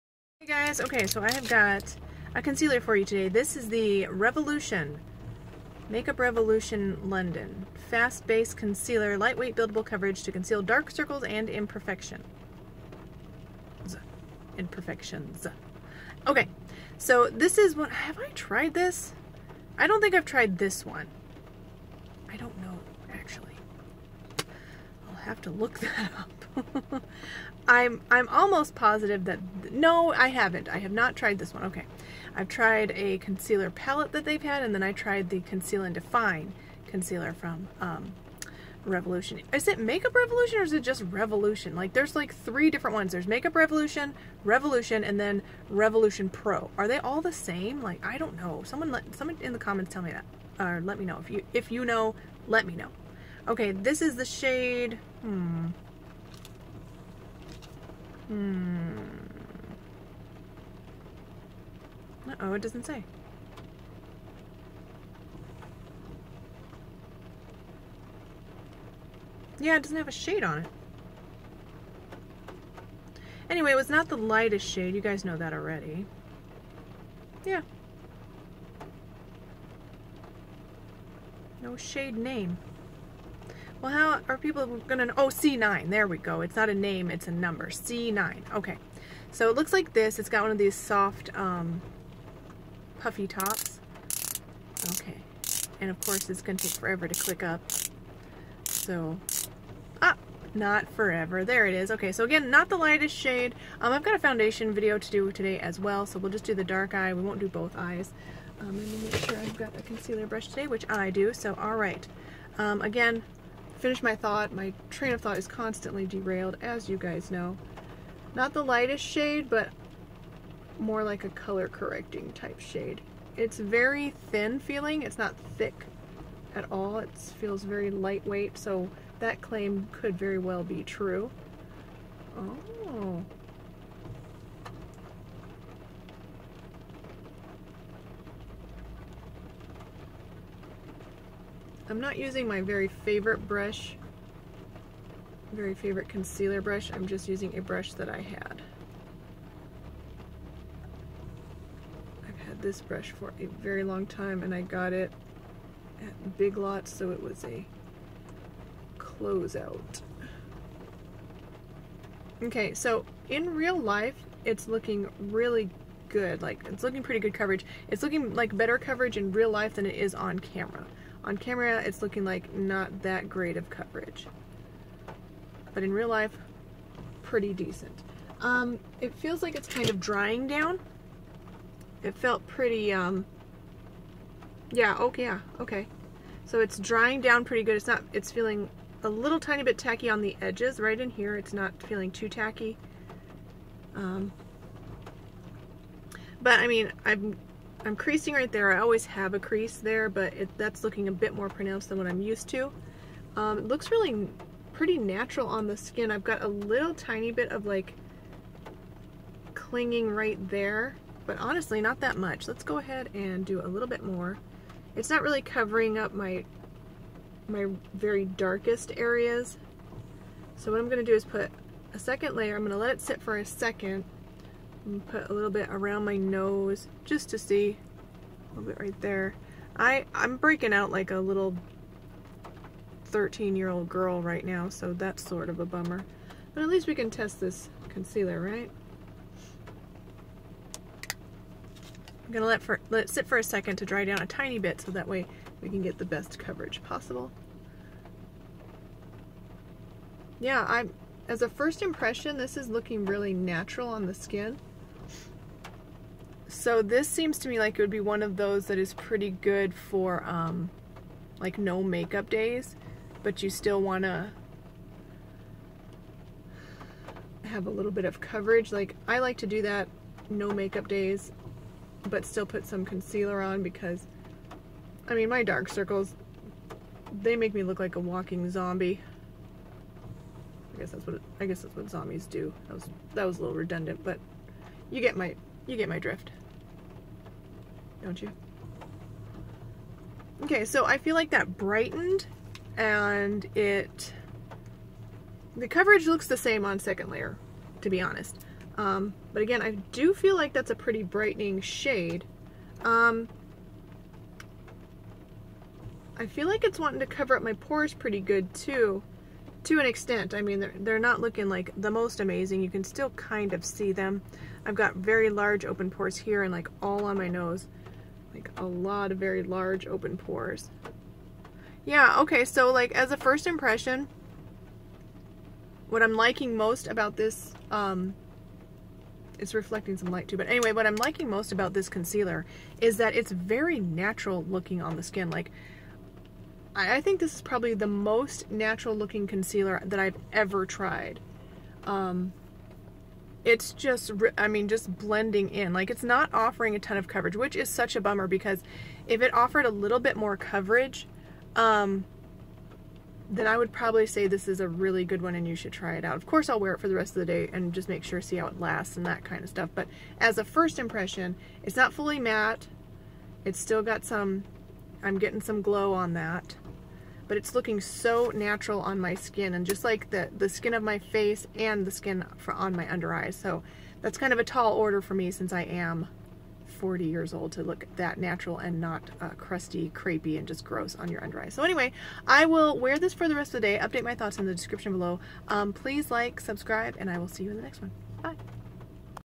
Makeup Revolution Fast Base Concealer review▪demo▪first impression▪over 40 makeup
*UPDATE*

This concealer was really good. It's medium  coverage and very natural on the skin. It's also very affordable at $5.